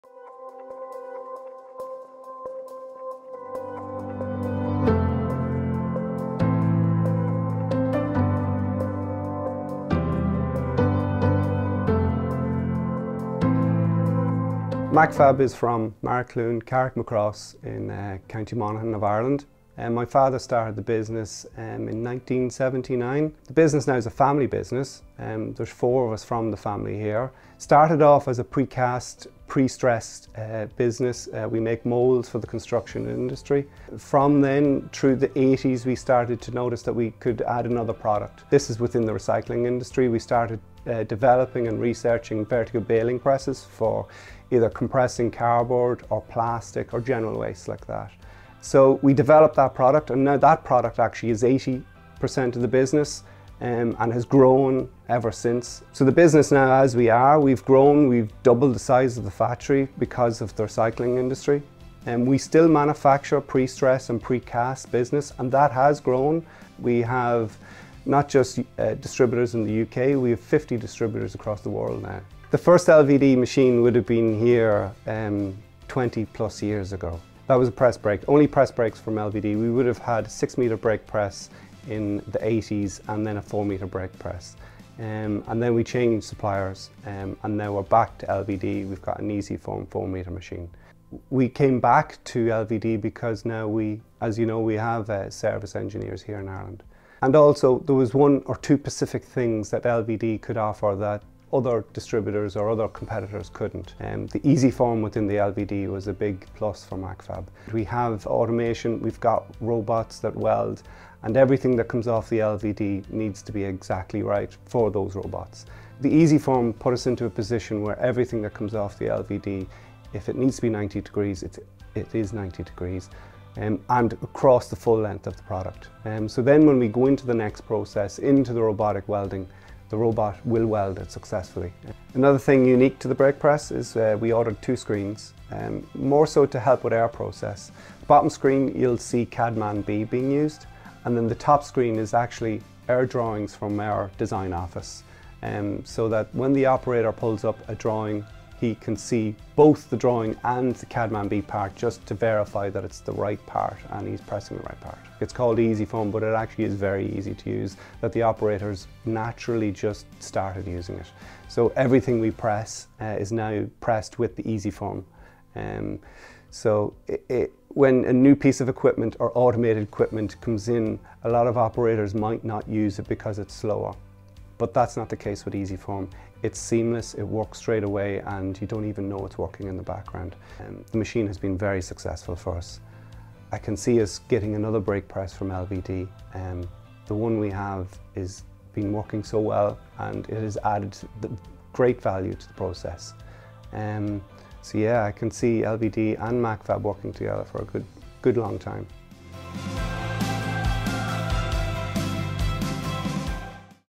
LVD customer MACFAB (Ireland)
An LVD Easy-Form press brake is giving MACFAB Systems Ltd the reliable accuracy it needs for the robot welding of high strength steel fabrications.